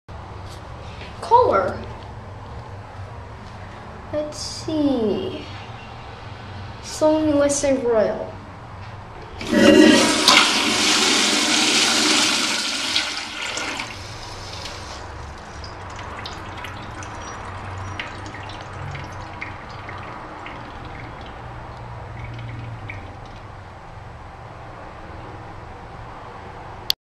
2008: 1994 Kohler Highcrest Toilet 2
This toilet is not original to the building, which was built in the early 1960's. This one has a 2000's Sloan Royal flushometer.